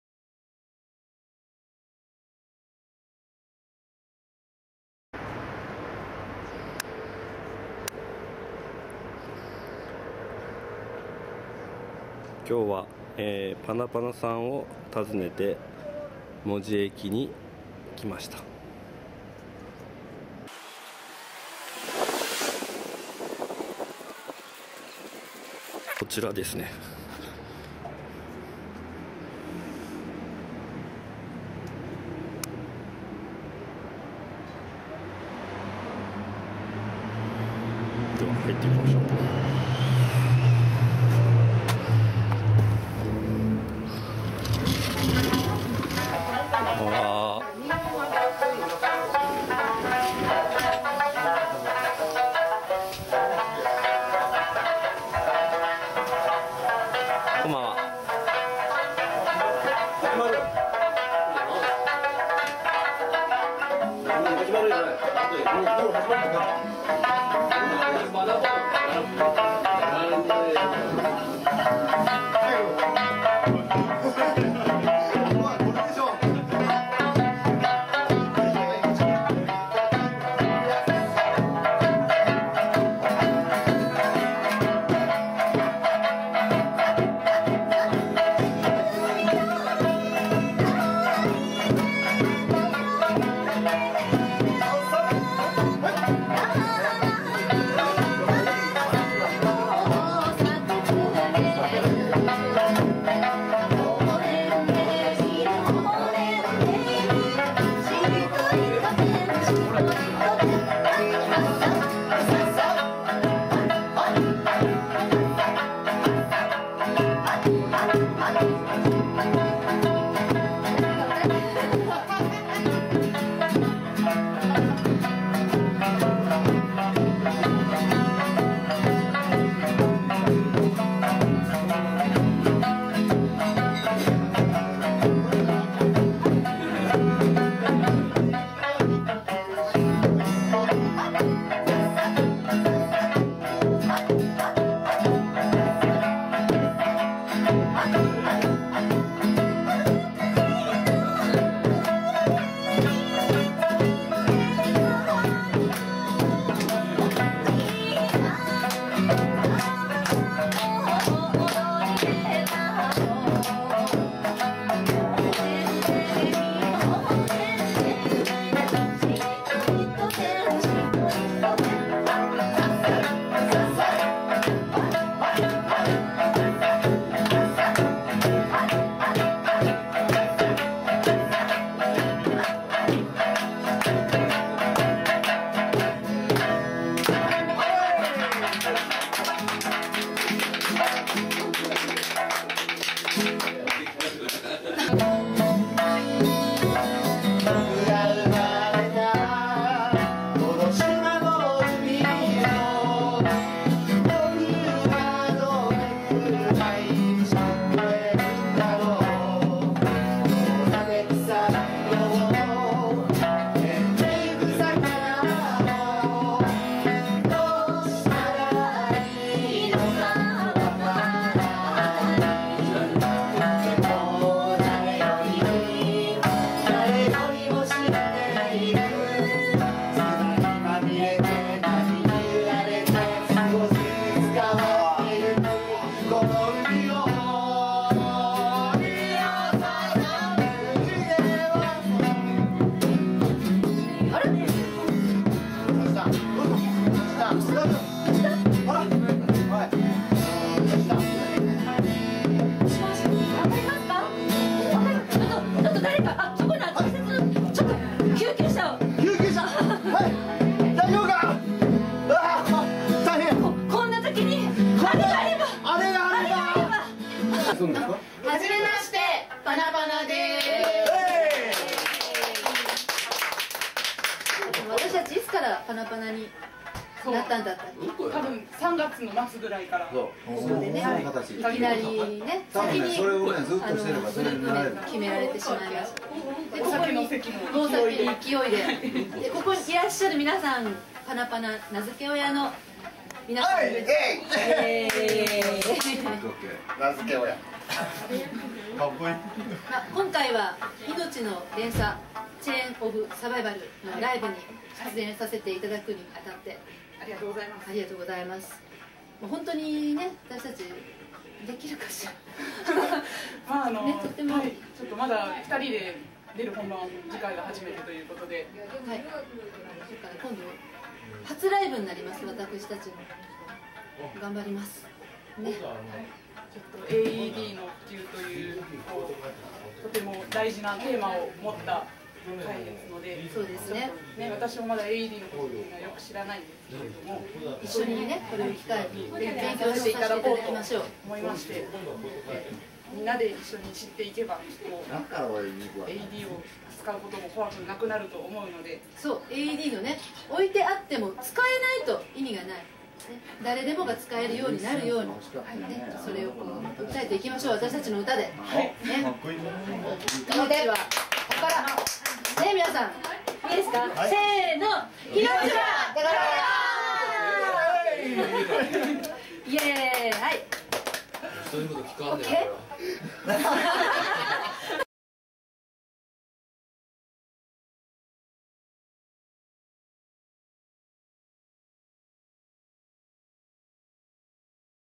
パナパナ / Chain Of Survival!!命の連鎖 vol.4
第4回「Chain Of Survival!!命の連鎖」のパナパナの コメント．音楽を通じて命の大切さを考えるイベントです．